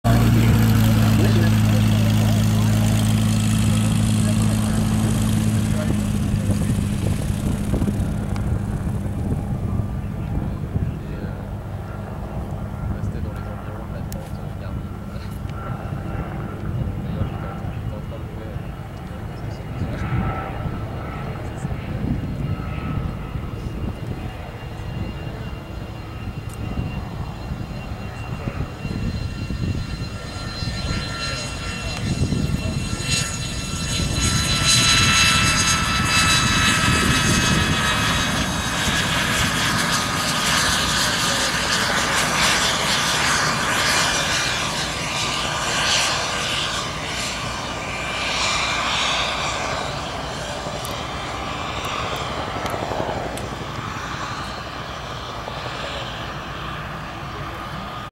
7/27/16 Aero L-39 Albatross landing!
An Aero L-39 Albatross lands at Oshkosh before the start of the Wednesday Airshow!